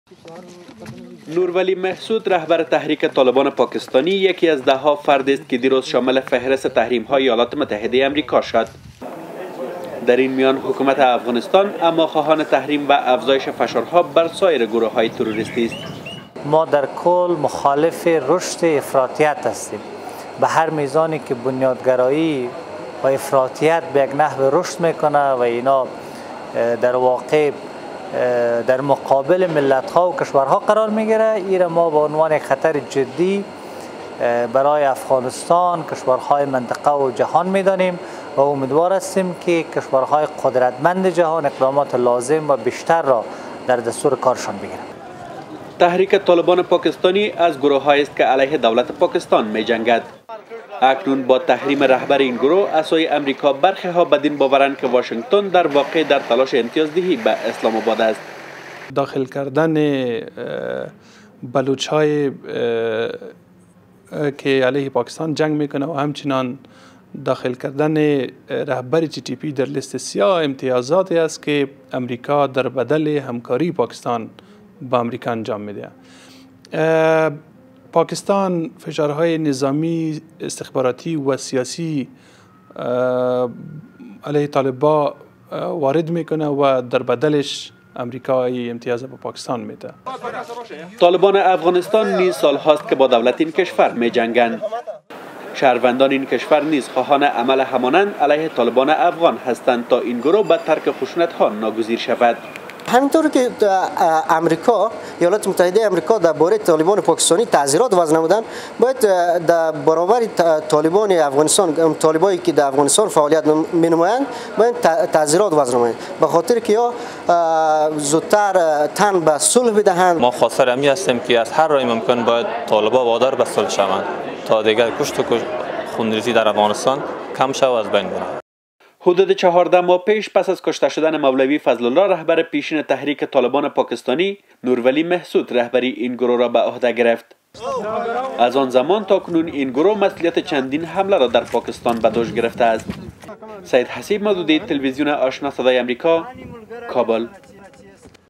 Afghanistan calls on sanction against Pakistani terror groups - VOA Ashna
بدنبال وضع تحریم ها بر رهبر تحریک طالبان پاکستانی از سوی امریکا،‌ حکومت افغانستان می گوید  که سایر گروه های تروریستی نیز بدون تفکیک باید تحریم شوند. با این حال، برخی از تحلیلگران بدین باورند که امریکا با تحریم تحریک طالبان پاکستانی، اسلام آباد را ازبهر کمک با روند صلح افغانستان، امتیاز می دهد_

دیدنی دیگر را تماشا کنید
خبرهای تلویزیون آشنا را از وبسایت، فیسبوک، تویتر و انستاگرام ما نیز دنبال کرده می توانید: